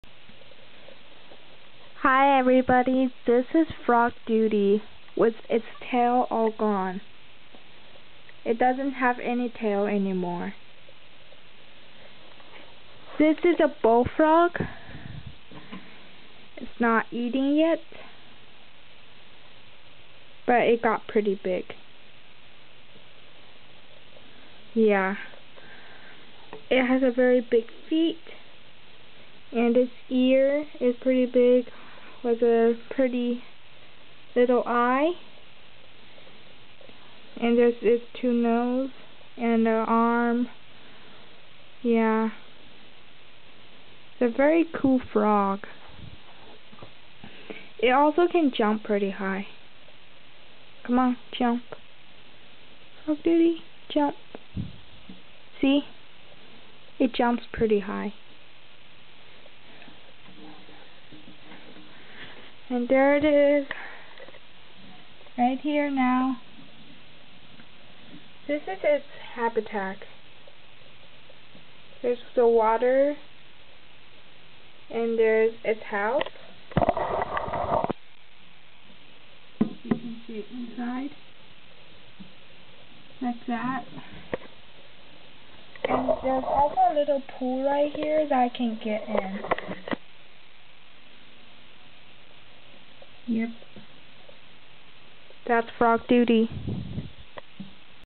Frogdudy the bullfrog
This is my little bullfrog name frogdudy,It is almost 2 years old now. I also need your help becasue it won't eat anything ! It won't eat mealworms, crickets, bugs, fly. It just won't eat !And yes it did lost its tail a few days ago.